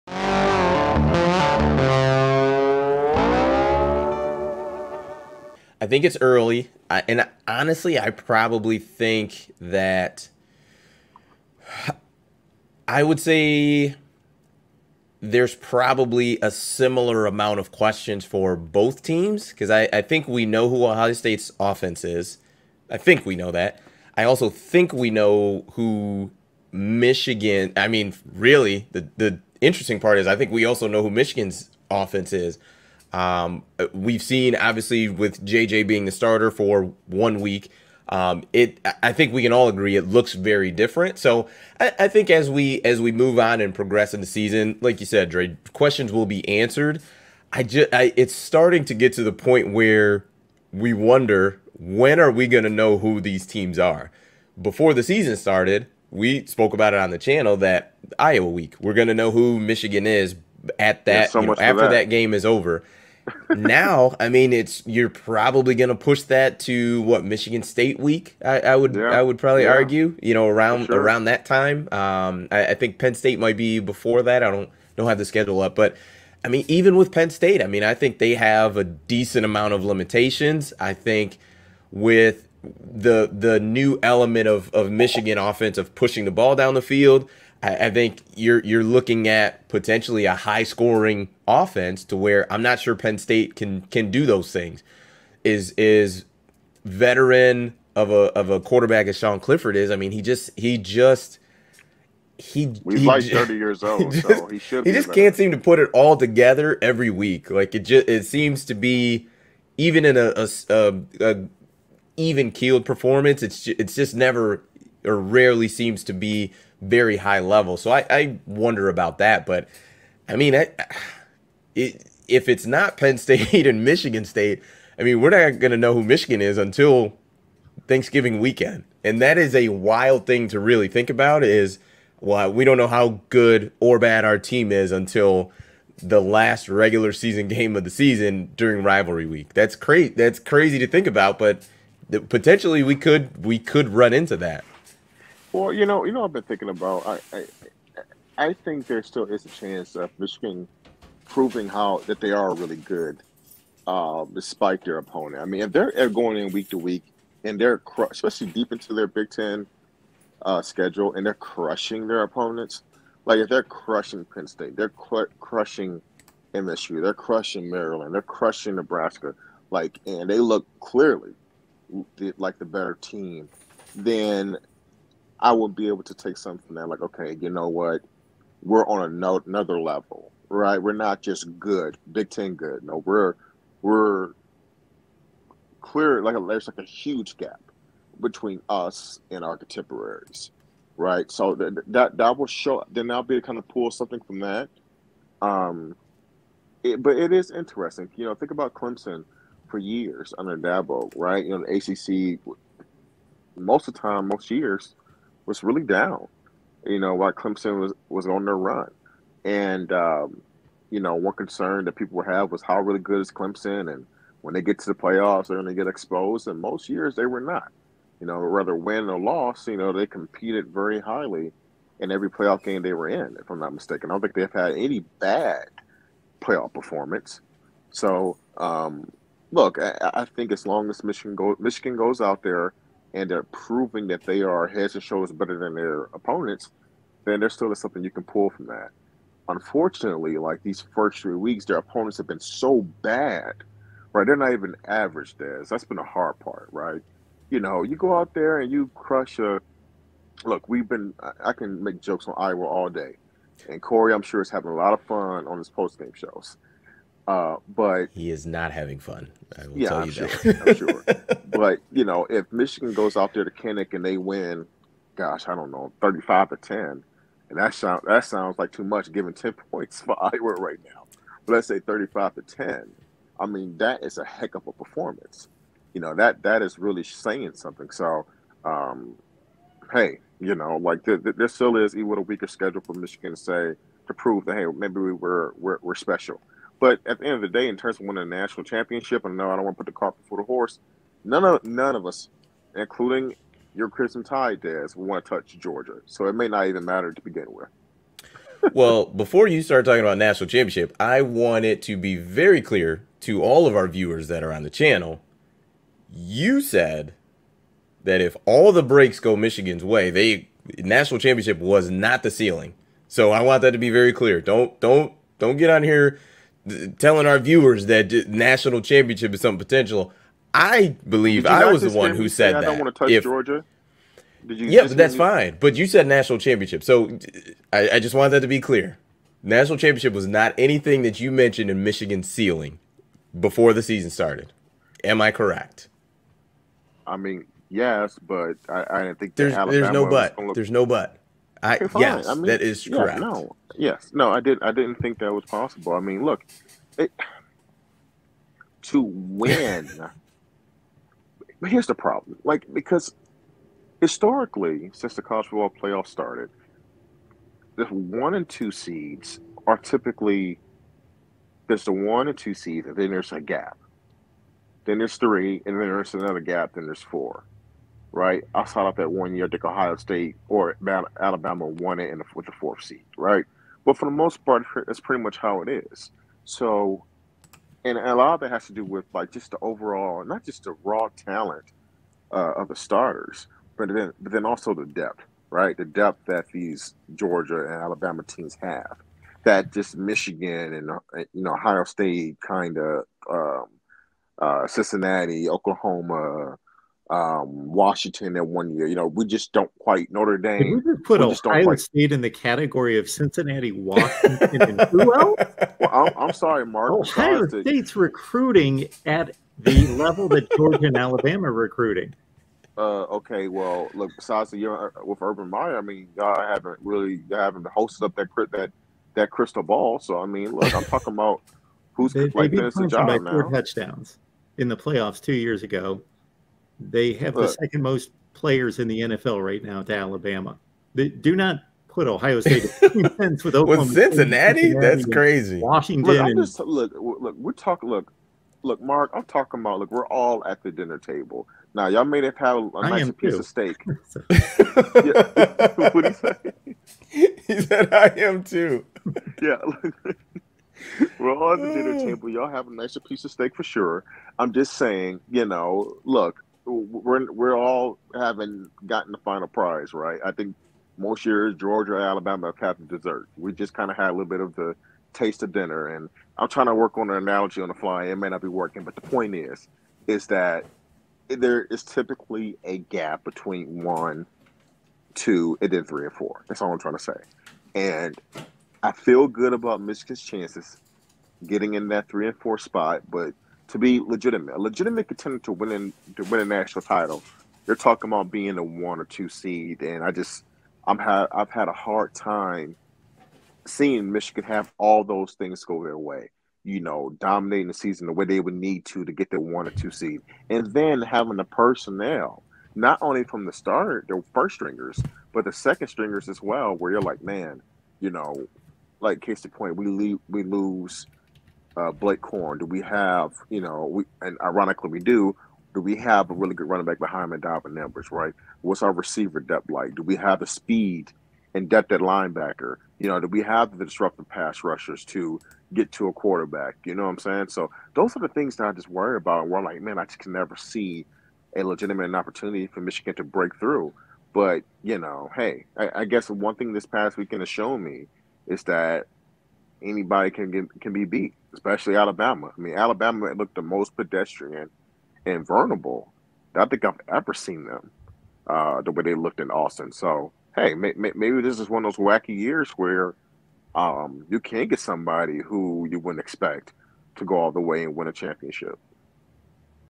Will QB Decision Help Michigan Focus Moving Forward?
Now that JJ McCarthy is the apparent QB1 for Michigan, the Wolverines can start to focus more attention on their future conference opponents and getting back to the playoffs.